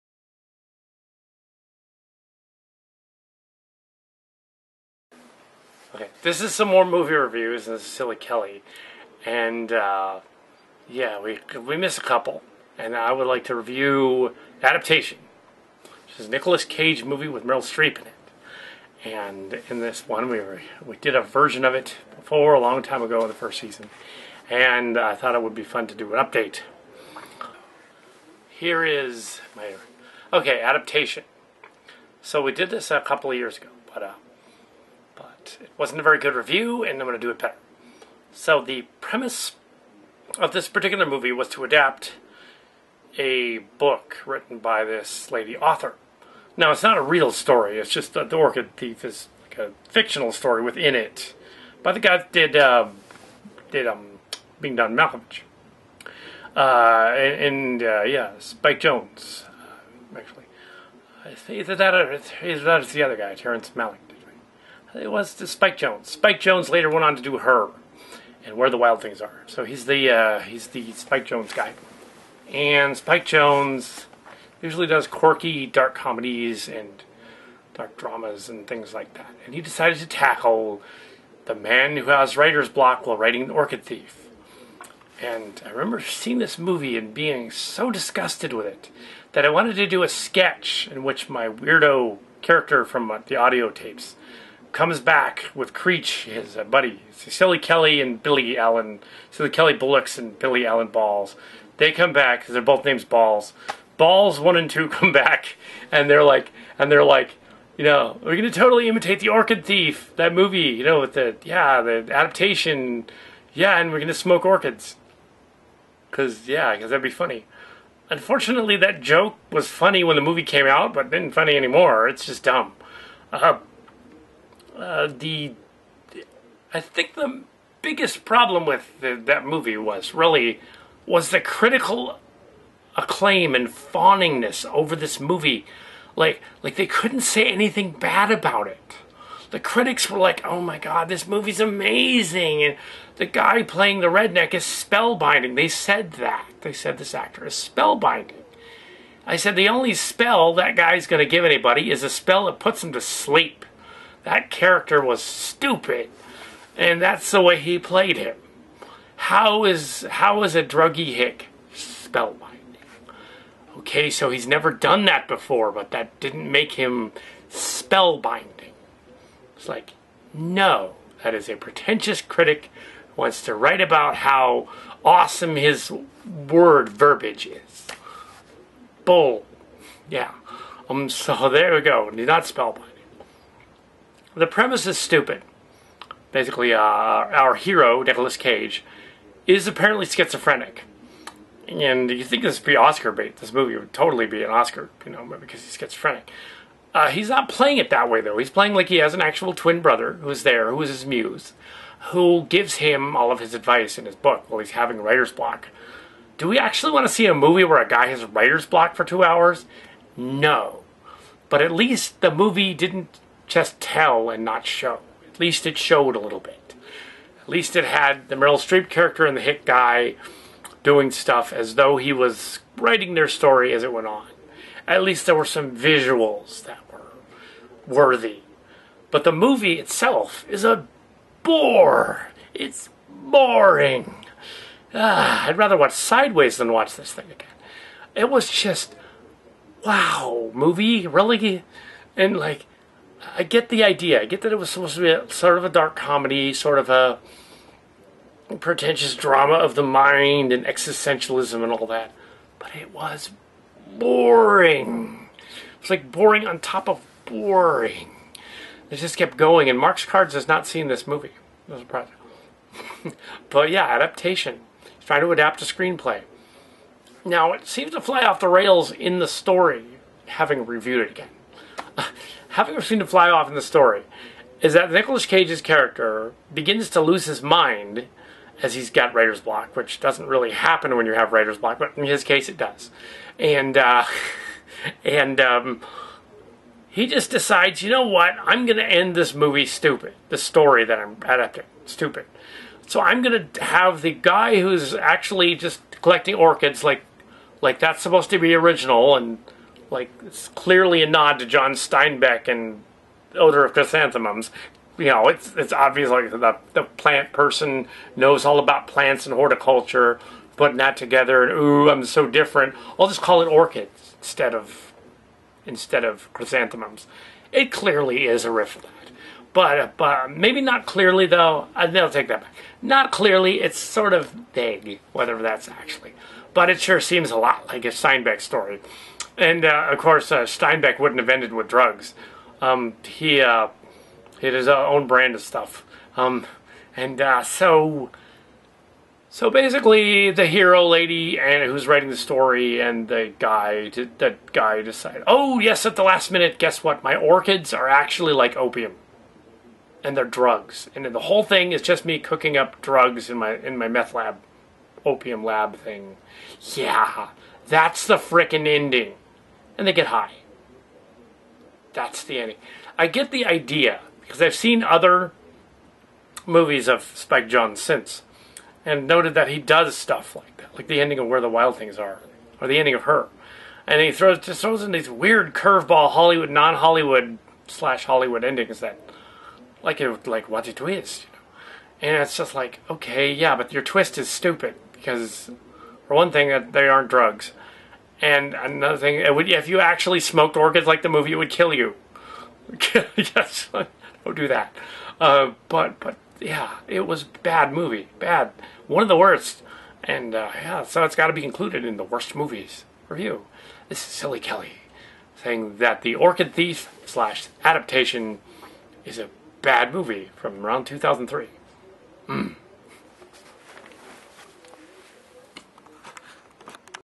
Kal Kat Adaptation movie review
Spike Jonze's somber look into the life of a writer who is trying to adapt The Orchid Thief (sort of like Odor of Chrysanthemums) and his writer's bock, is a pretentious stolid bore. Critics fawned over this, called characters spellbinding, and the story incredible, and wanted to give it Oscars.

Kelly calls it overblown, boring, and crazy, and the idea of the last 20 minutes being a reveal that the orchids were drugs is a dumb tacked on ending nobody seems to mind! (They're not drugs. You could die from eating orchids). So annoyed by this movie, he had gone on to see other similar Jonze films, and is still not impressed. This movie still sucks.

Nicholas Cage doesn't help being the lead. It's a vanity piece though as Jonze and Cage's cousin were married then, that is Sophia Coppella. This movie sucks!